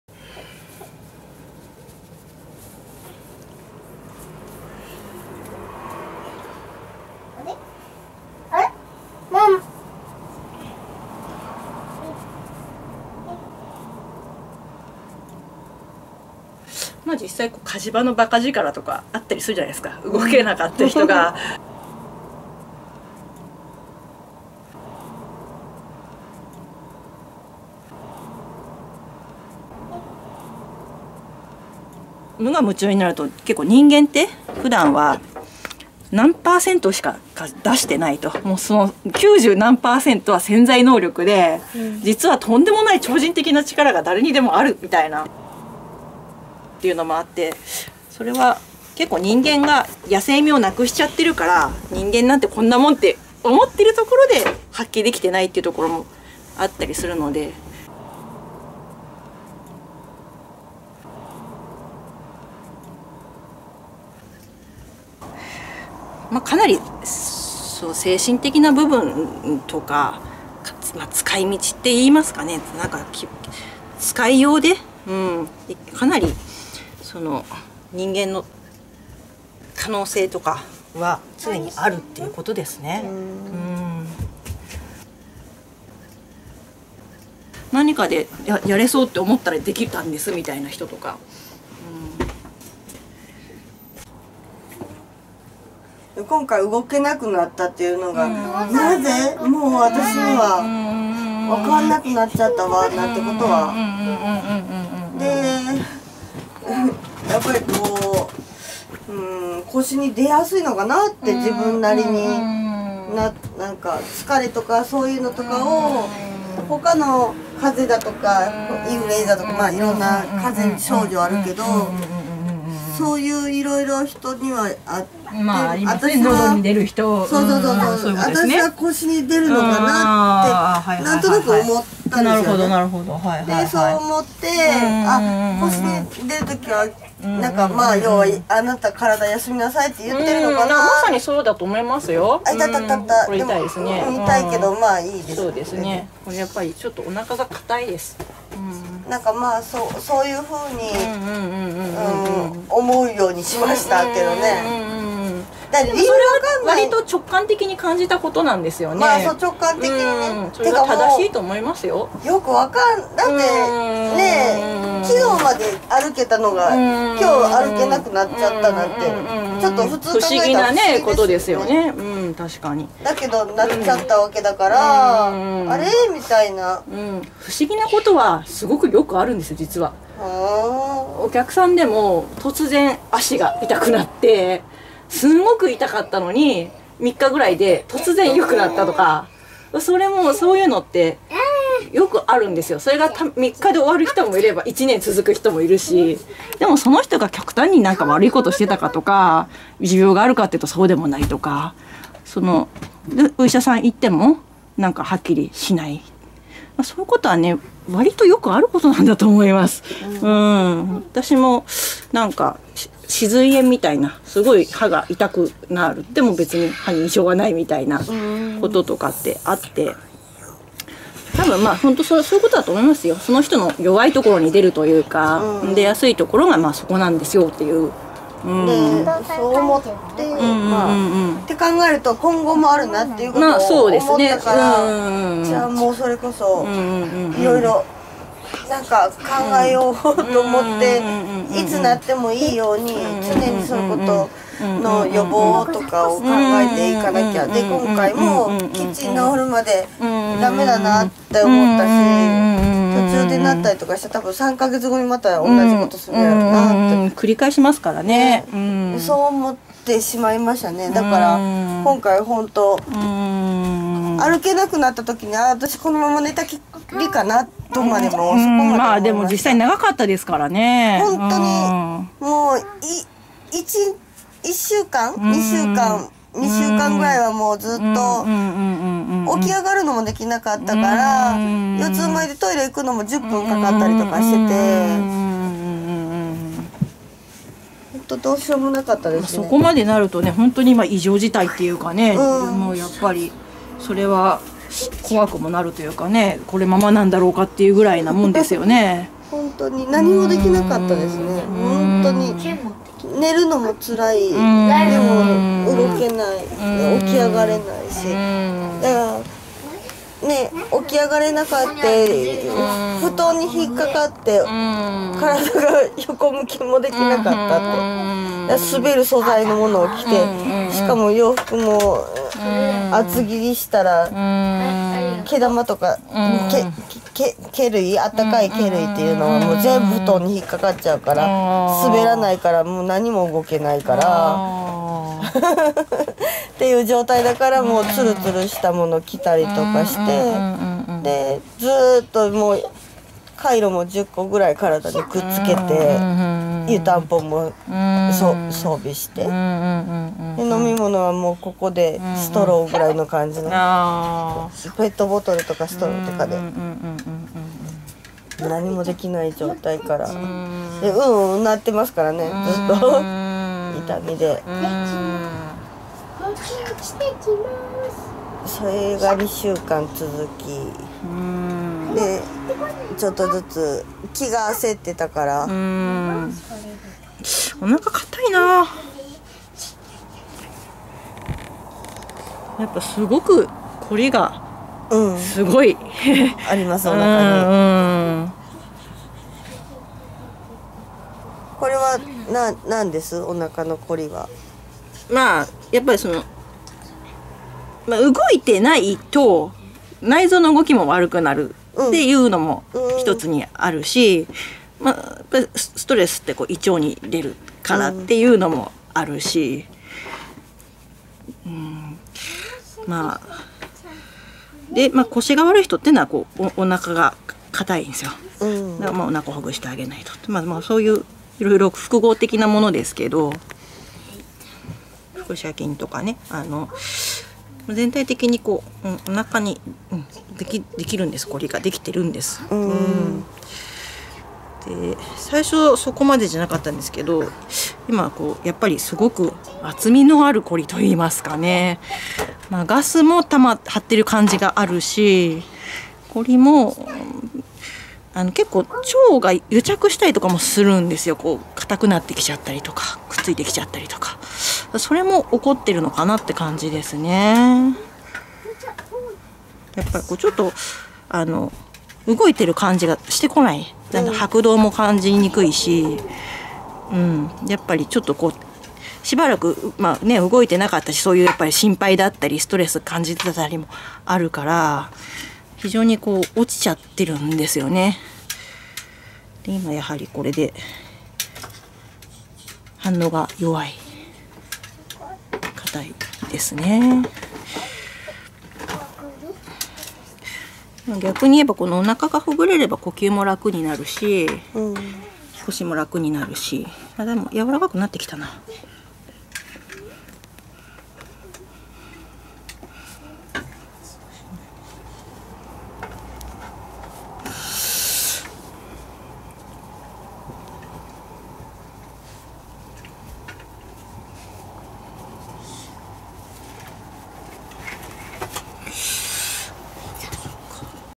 【腕～胸/おなか】腰痛エミさんにストレッチ＆腸揉み整体２【りらく屋】
の続きです。お話しながらの施術です。